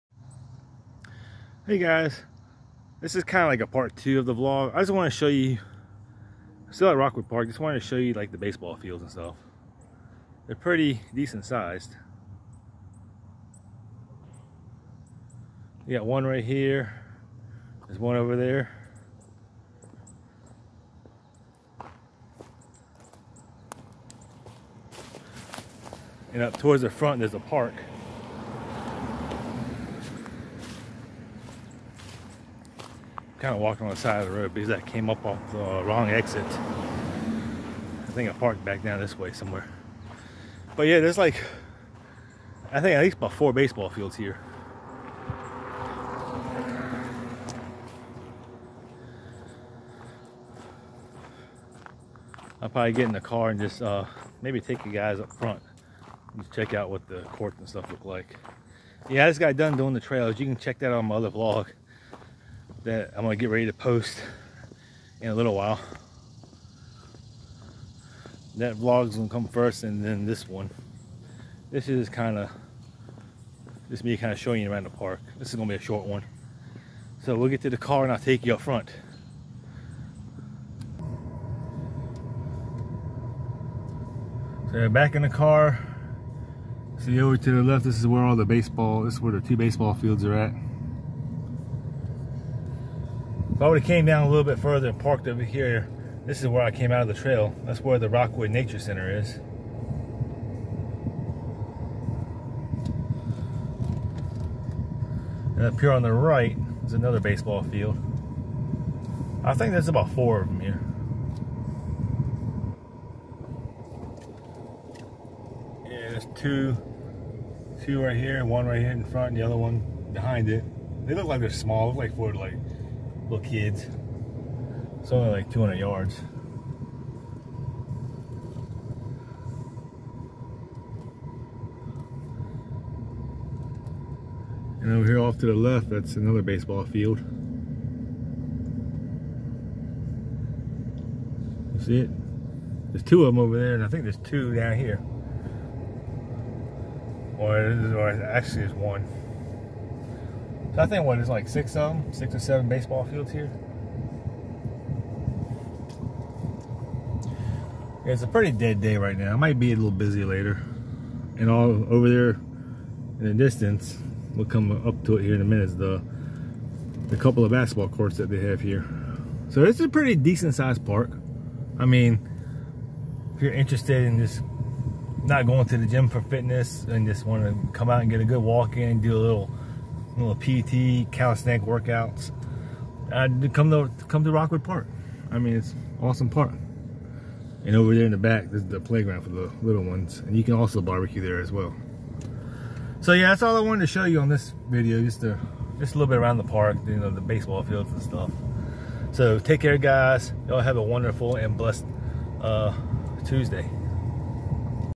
Rockwood Park Revealed: Unveiling the Exciting World of Softball, Basketball, and More!
Just showing around the rest of Rockwood Park. Pt 2. #fitness